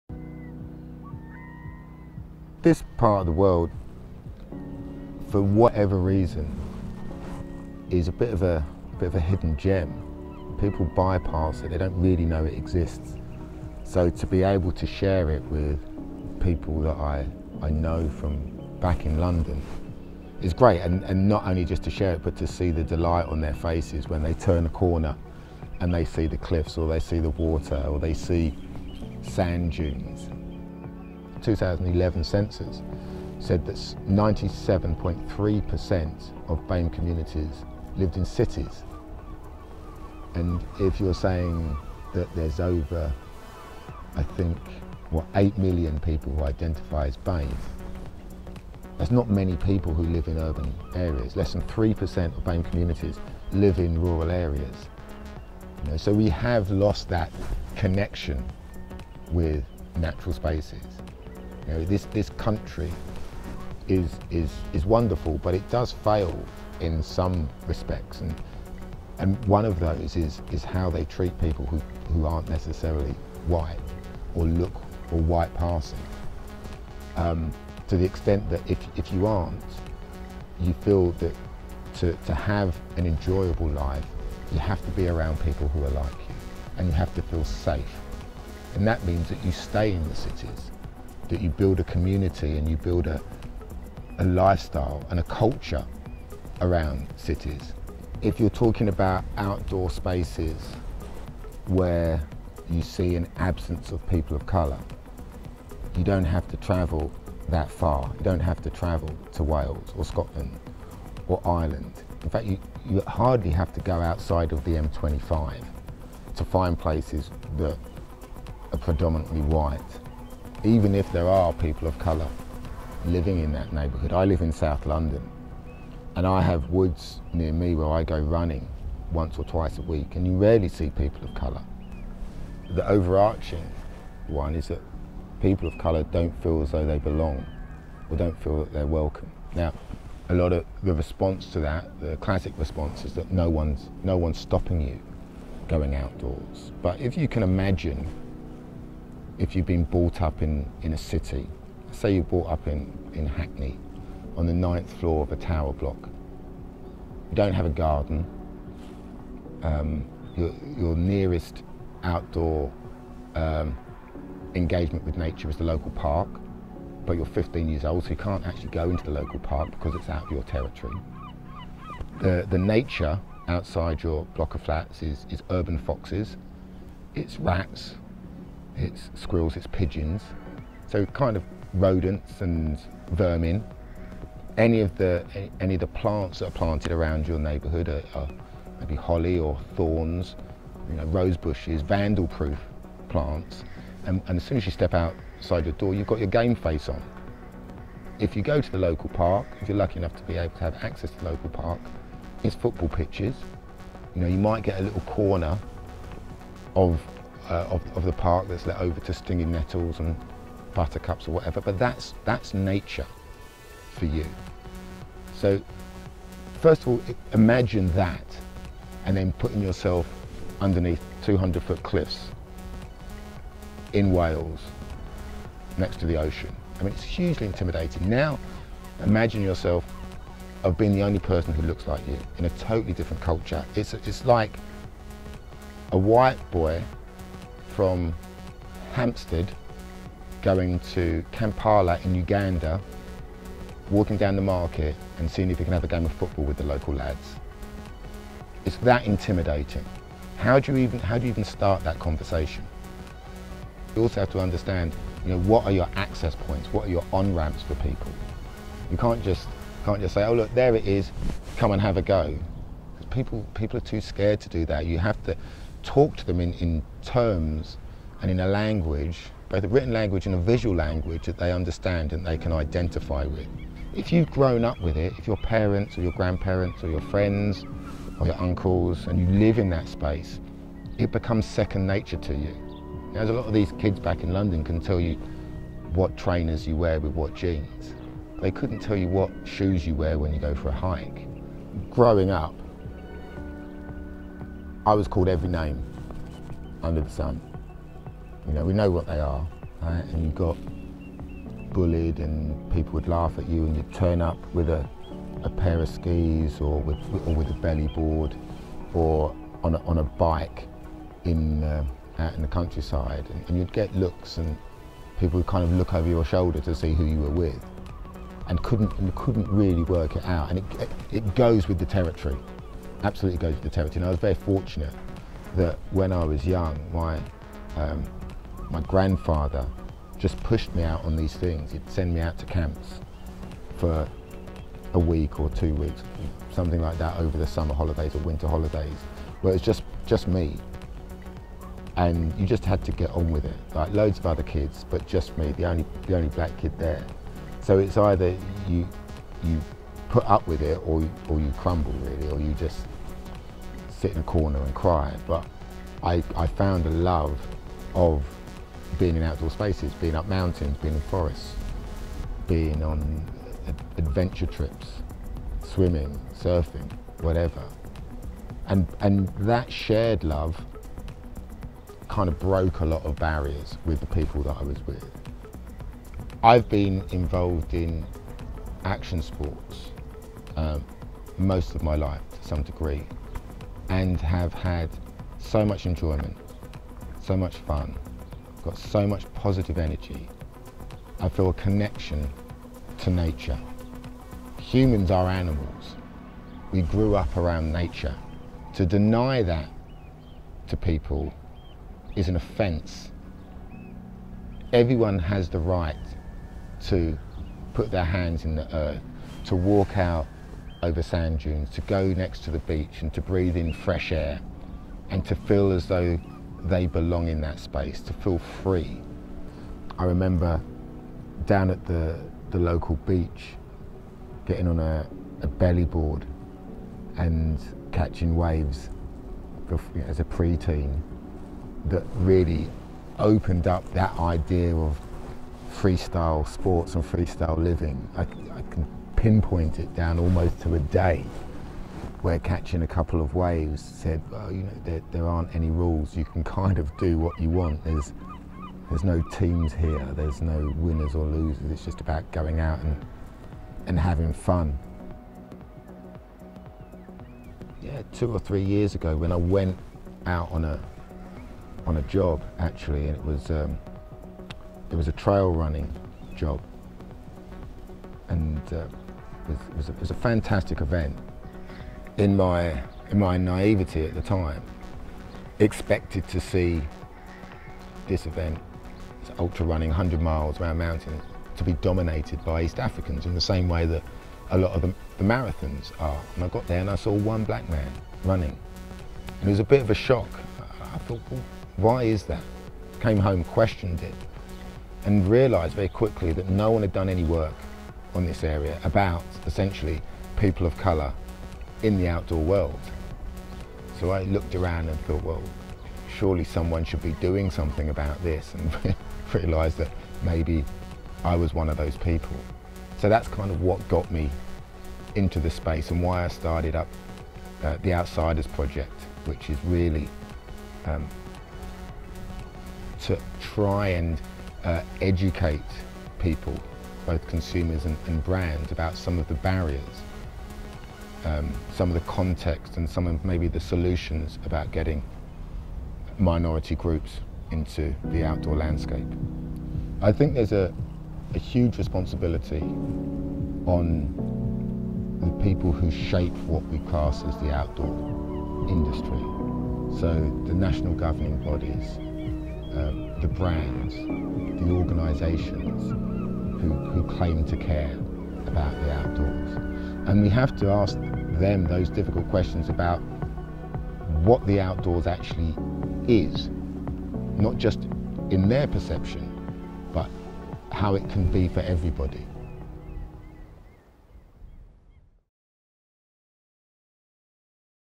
Phil Young: New Lenses on the Landscape | Ocean Openers
"You feel that, you have to be around people that are like you, and you have to feel safe. And that means that you stay in the cities, and you build a community, and you build a lifestyle and culture around cities." - Phil Young, founder of the Outsiders Project, has used his extensive background in action sports to become an advocate for diversity in the outdoors.

Film by Dan Magee
Photography by David Gray